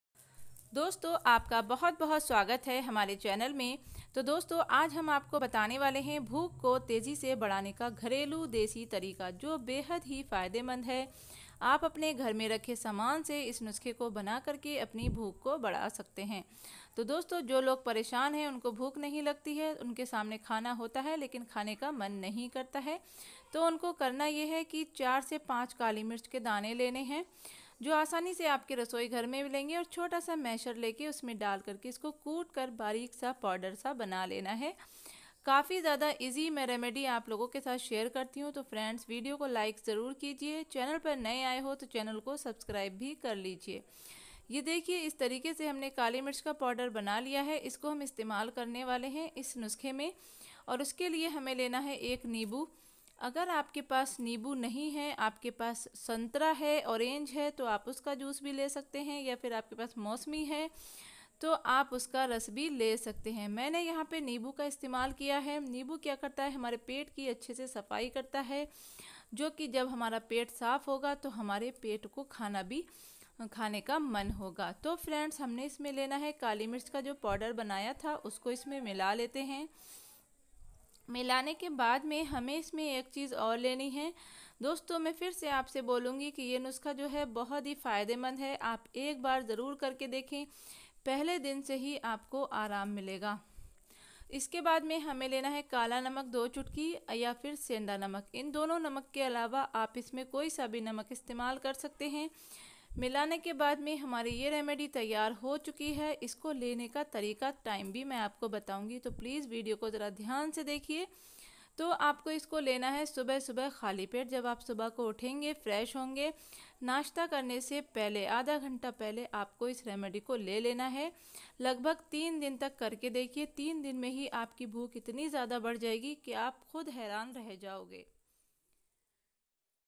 भूख बढ़ाने का रामबाण देसी नुस्खा, bhookh badhane ka gharelu upay, increase diet
प्यारे दोस्तों आज इस वीडियो में हमने बताया है कि अगर आपको भूख नहीं लगती है या आपके शरीर को खाया पिया नहीं लगता है तो आप इस वीडियो को देखकर के आसान से घरेलू नुस्खे को बनाकर इस्तेमाल करने के बाद आपको तेजी से भूख भी लगेगी आपका पेट भी अच्छे से साफ होगा दोस्तों जब हमें भूख नहीं लगती है तो हमारा पेट साफ नहीं होता है और इस नुस्खे से आपका पेट अच्छे से साफ होगा आपको खुलकर भूख लगेगी खाया पिया भी लगना शुरू हो जाएगा।।

धन्यवाद।